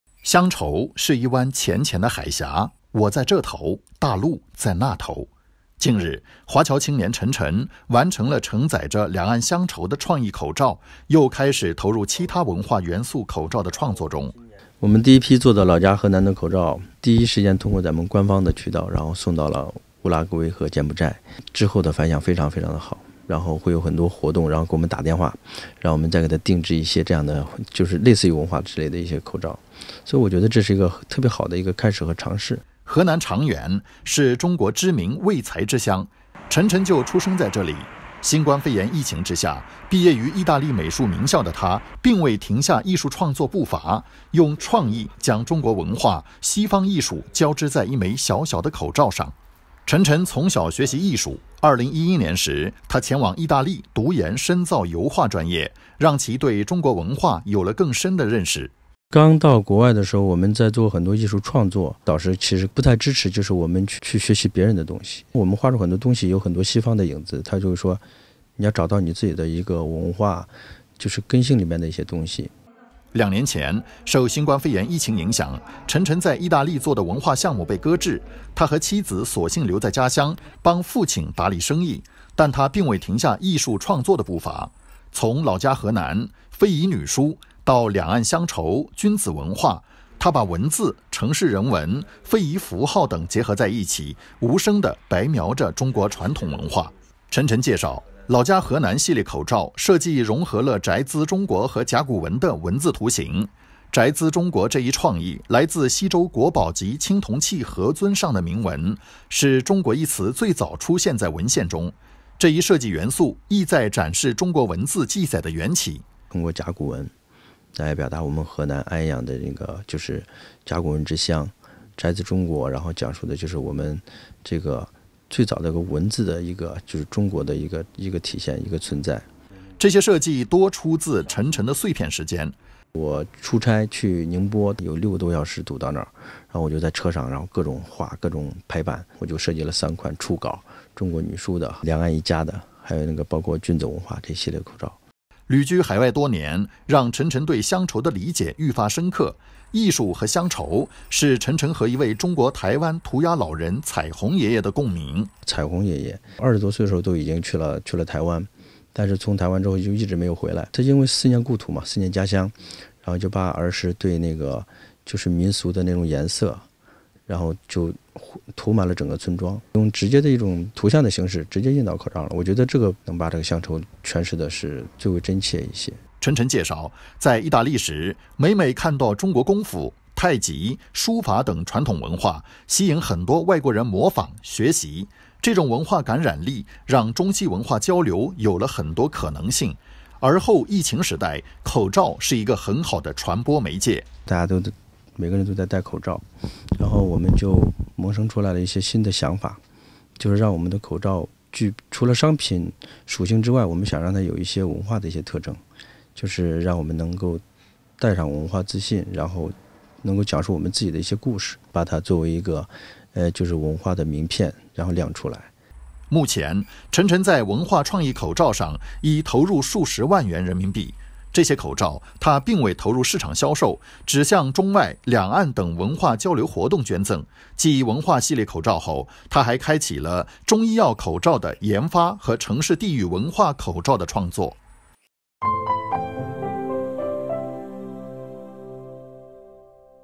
口罩上的文化与乡愁 侨青把口罩做成文化名片
“乡愁是一湾浅浅的海峡，我在这头，大陆在那头……”近日，华侨青年陈晨完成了承载着两岸乡愁的创意口罩，又开始投入其它文化元素口罩的创作中。

河南长垣是中国知名“卫材之乡”，陈晨就出生在这里。新冠肺炎疫情之下，毕业于意大利美术名校的他，并未停下艺术创作步伐，用创意将中国文化、西方艺术交织在一枚小小的口罩上。

陈晨从小学习艺术，2011年时，他前往意大利读研深造油画专业，让其对中国文化有了更深的认识。

两年前，受新冠肺炎疫情影响，陈晨在意大利做的文化项目被搁置，他和妻子索性留在家乡帮父亲打理生意，但他并未停下艺术创作的步伐。从“老家河南”、“非遗女书”，到“两岸乡愁”、“君子文化”，他把文字、城市人文、非遗符号等结合在一起，无声地“白描”着中国传统文化。

陈晨介绍，“老家河南”系列口罩，设计融合了“宅兹中国”和“甲骨文”的文字图形。“宅兹中国”这一创意来自西周国宝级青铜器何尊上的铭文，是“中国”一词最早出现在文献中，这一设计元素意在展示“中国”文字记载的源起。

这些设计多出自陈晨的碎片时间。旅居海外多年，让陈晨对乡愁的理解愈发深刻。艺术和乡愁，是陈晨和一位中国台湾涂鸦老人“彩虹爷爷”的共鸣。

陈晨介绍，在意大利时，每每看到中国功夫、太极、书法等传统文化，吸引很多外国人模仿、学习，这种文化感染力，让中西文化交流有了很多可能性。而后疫情时代，口罩是一个很好的传播媒介。

目前，陈晨在文化创意口罩上已投入数十万元人民币。这些口罩，他并未投入市场销售，只向中外、两岸等文化交流活动捐赠。继文化系列口罩后，他还开启了中医药口罩的研发和城市地域文化口罩的创作。（李超庆 阚力）